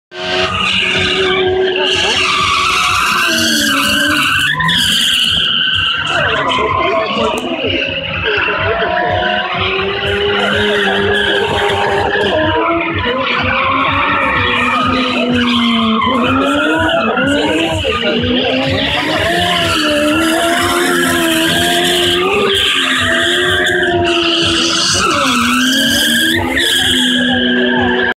RWD swapped Kei-cars playing at Mt. Ibuki
2017 Team Tetsujin event